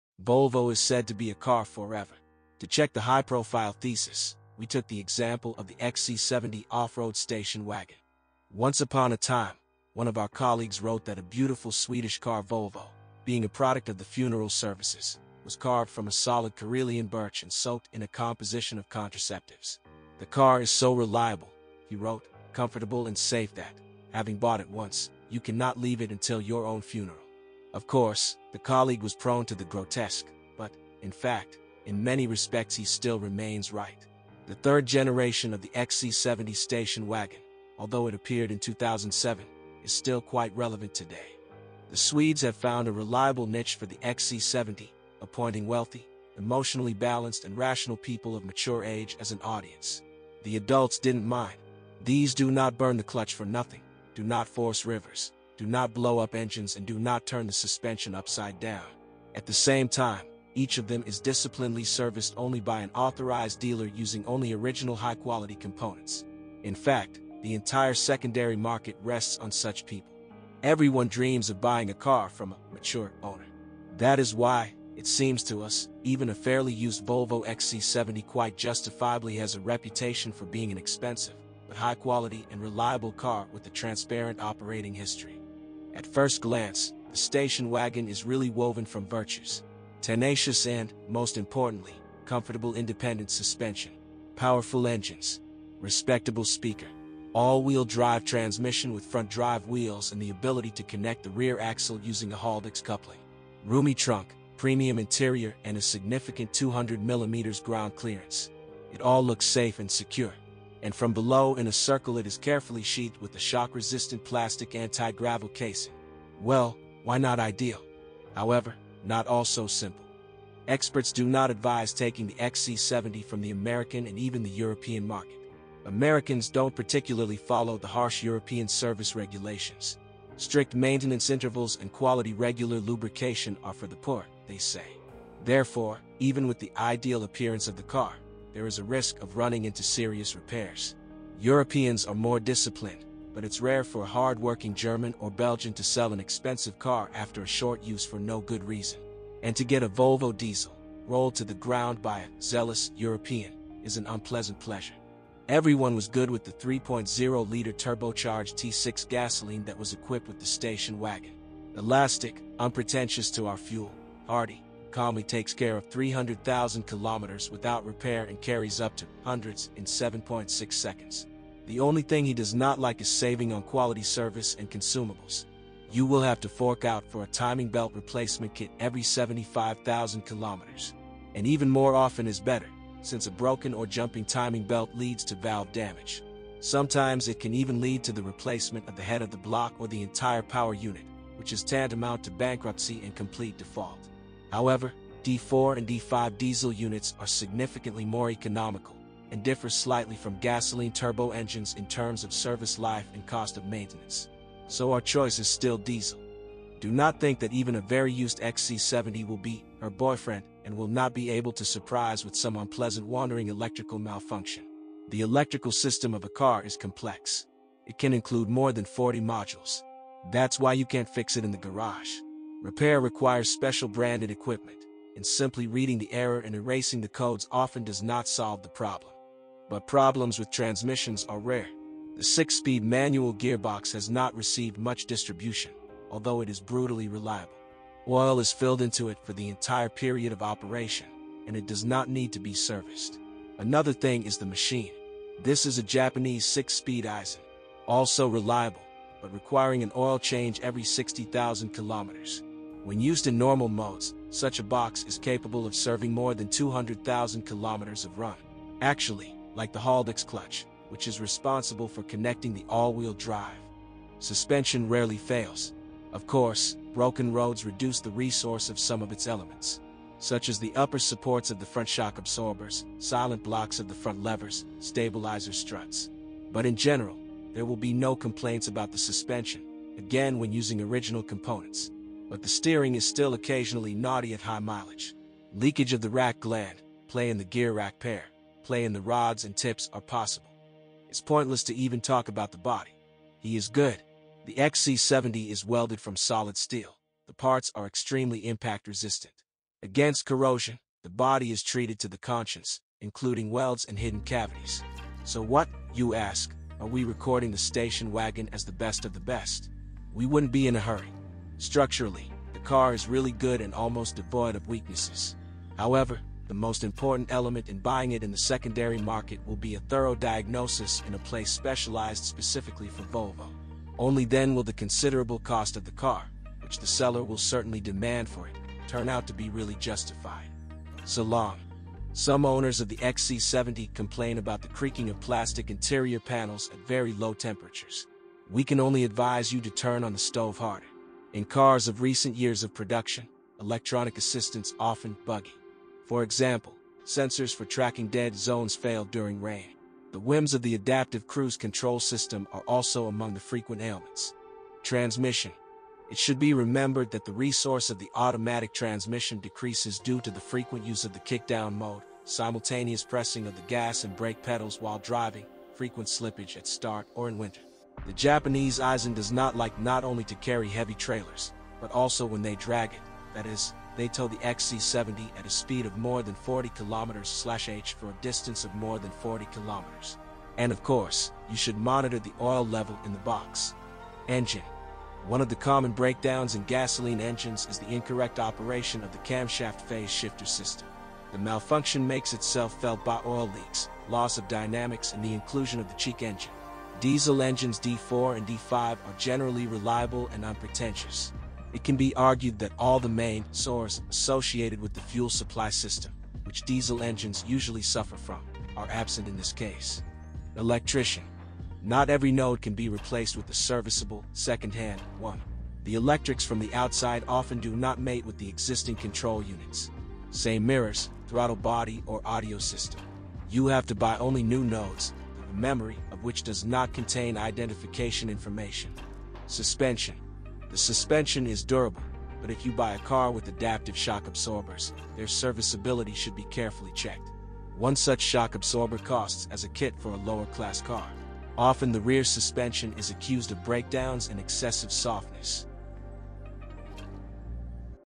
Volvo XC70 2 Problems | Weaknesses of the Used Volvo XC70 II
Should you buy a used Volvo XC70 II? Pros and cons, maintenance costs and common problems of the Volvo XC 70 2.

Transmission problems
Engine reliability